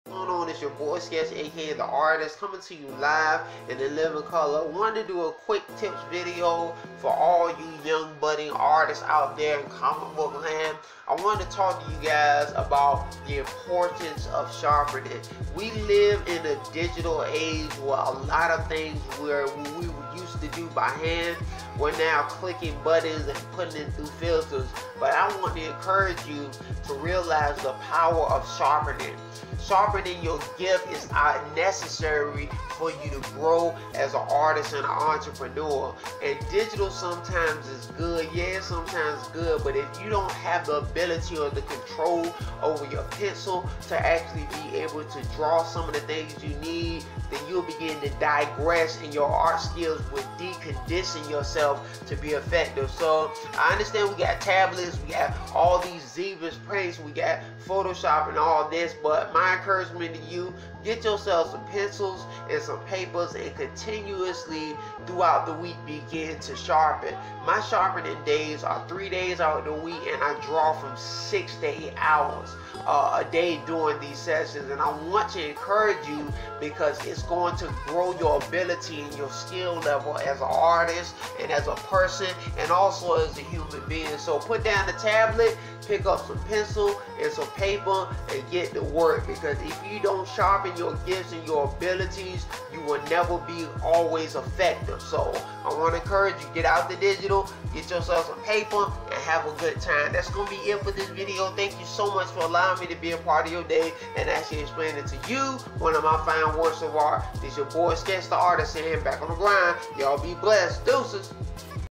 Faith Blade University the power of Sharpening
Just reminding you guys making sure you practice on your gift. Technology can sometimes make you lazy. Even though it is old fashioned try drawing on paper. If you do not draw. It can affect your skills as an artist.. Always choose at least 2 times out of the week to sharpen and practice..